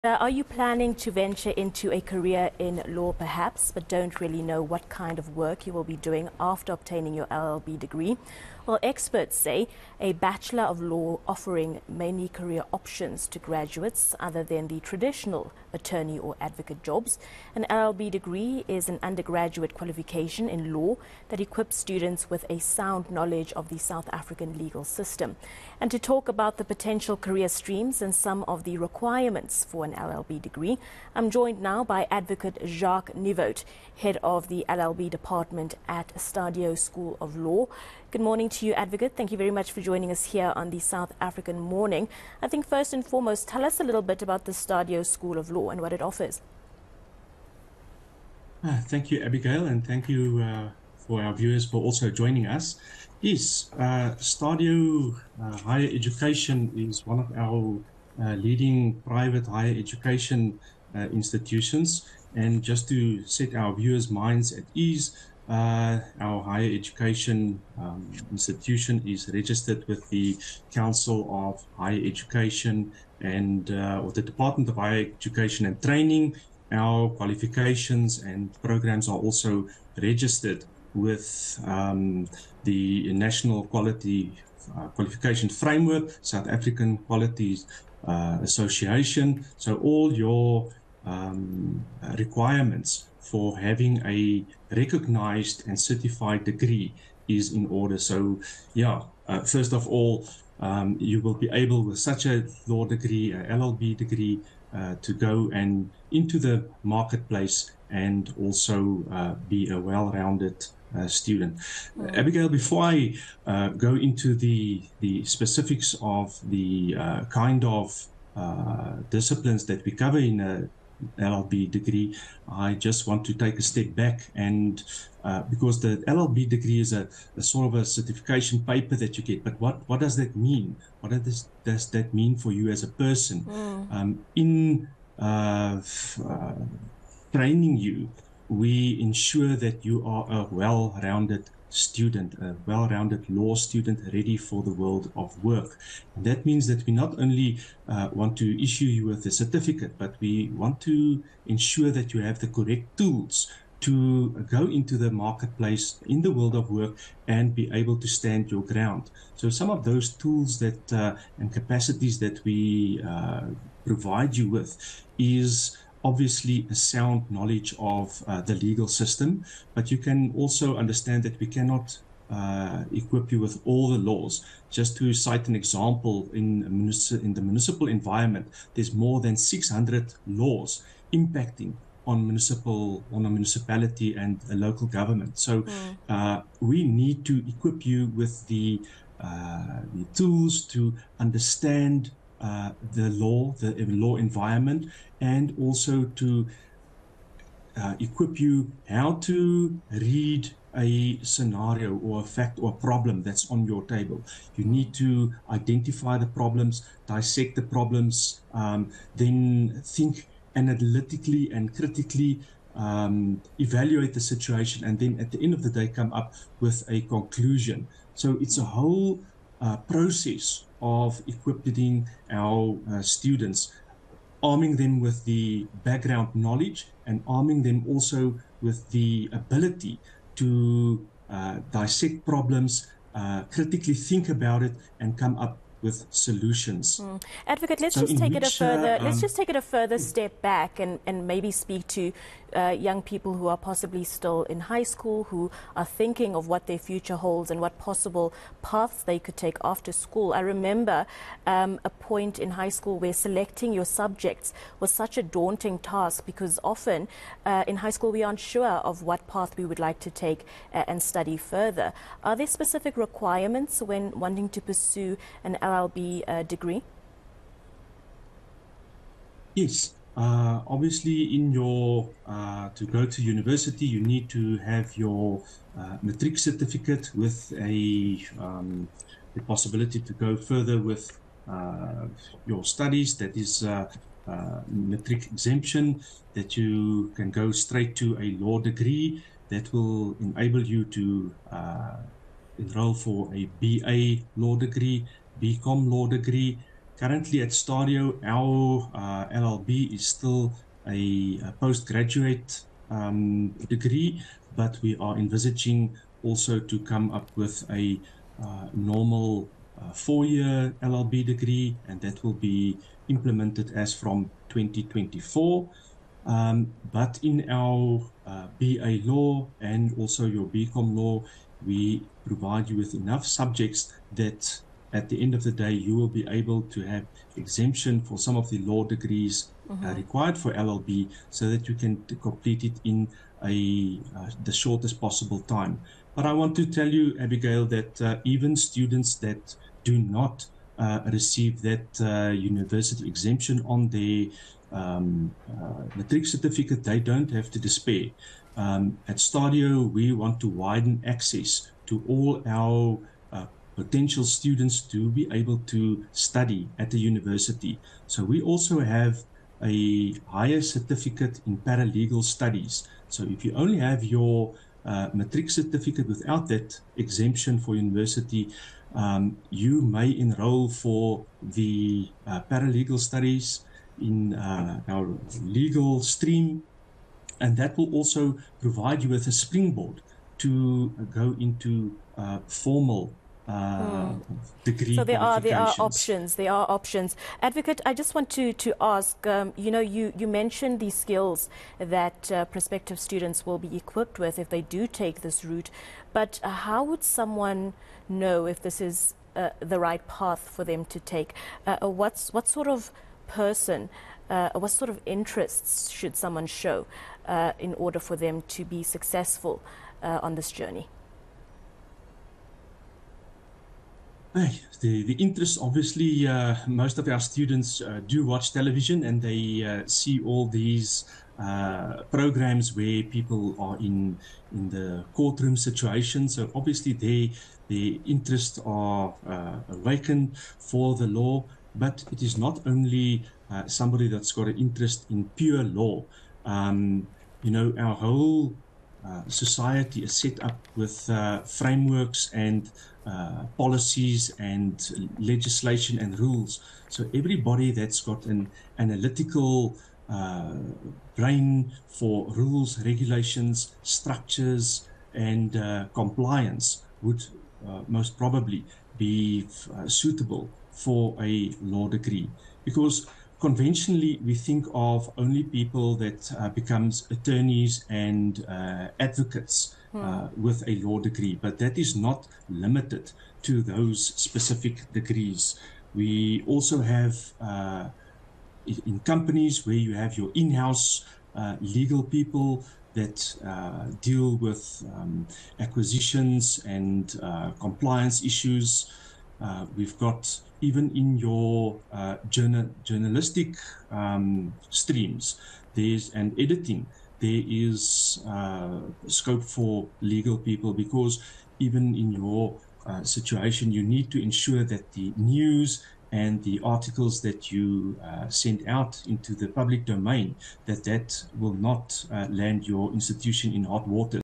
Discussion | Career options in law
eNCA speaks to Advocate Jacques Nieuwoudt, Head of the LLB Department at STADIO School of Law.